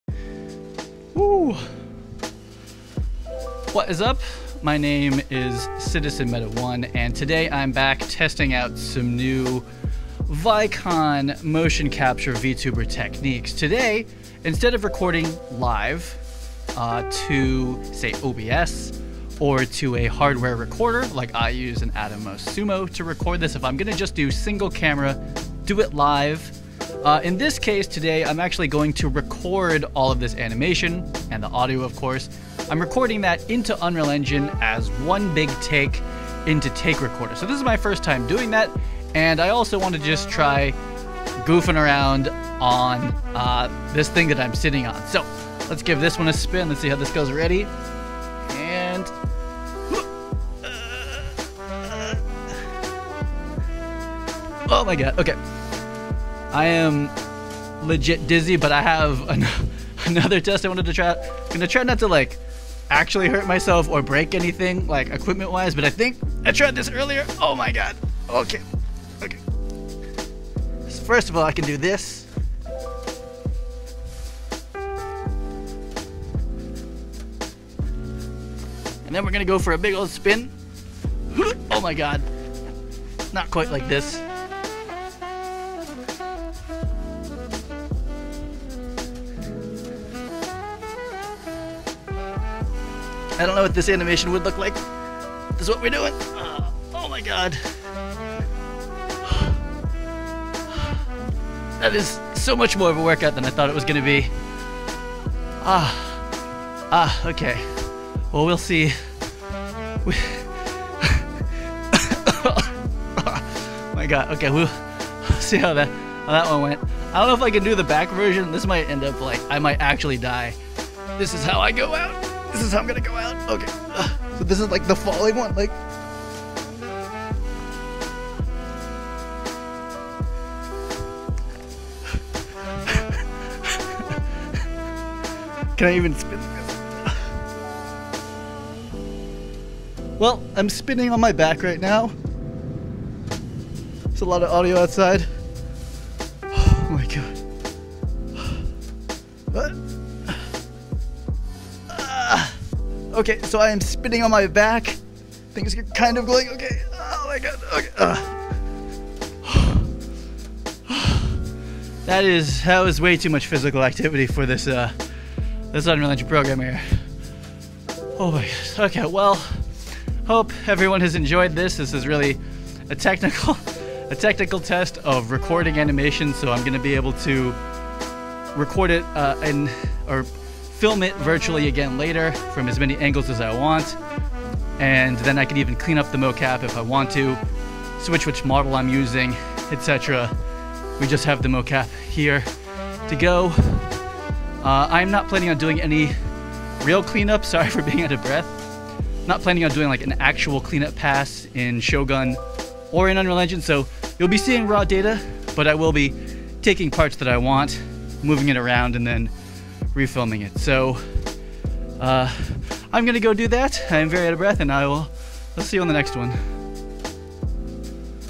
The Invisible Stool - Indie Vicon VTuber
This is one continuous raw clip using a Vicon motion capture system (3 finger).  I recorded the face and body data using Take Recorder in Unreal Engine 4.27 and rendered it in 4K using Movie Render Queue.

Notes:
- The face tracking is not very good in this demo.  I probably needed to recalibrate Live Link Face.
- The animation was assembled in Sequencer and it's possible that the face animation needs a different setting for retargeting.  Not sure.
- Same issue with elbows as the last demo, I have to speak with Vicon and Epic Games on the best way to approach this.
- Small detail is that the hands are not being position constrained, so when the contact the floor they go through it, which in this demo is mostly fine.

Computer:
HP Z8 - Dual Intel Xeon Gold - 192 GB RAM - Nvidia A600

This sequence took about 2 hours to render at 4K 24FPS.